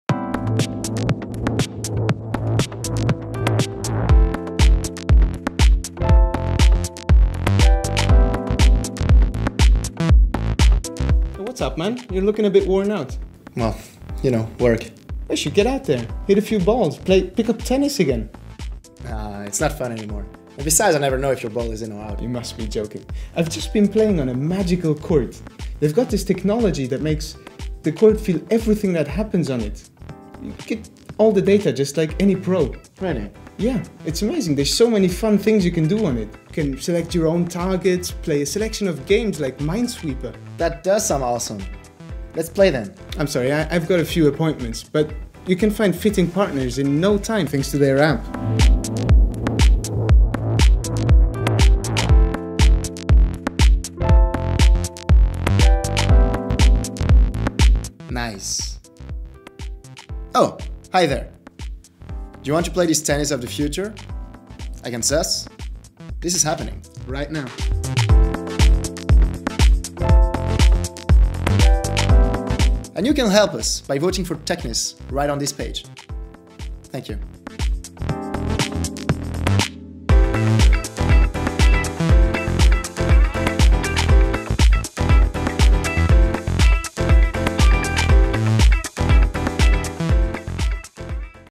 TECHNIS
TECHNIS hat eine neue Generation von Tennisplätzen entwickelt, die berührungsempfindlich sind und verstehen was auf der Oberfläche geschieht. Mit Hilfe dieser neuen Technologie – die auch für andere Sportarten eingesetzt werden können – können Technik und Sport in einer integrierten und nahtloser Art und Weise kombiniert werden. Dies ermöglicht beispielsweise die Position des Balles zu prüfen, die Bewegungen der Spieler zu verfolgen und die ganzen Spielstatistiken zu sammeln.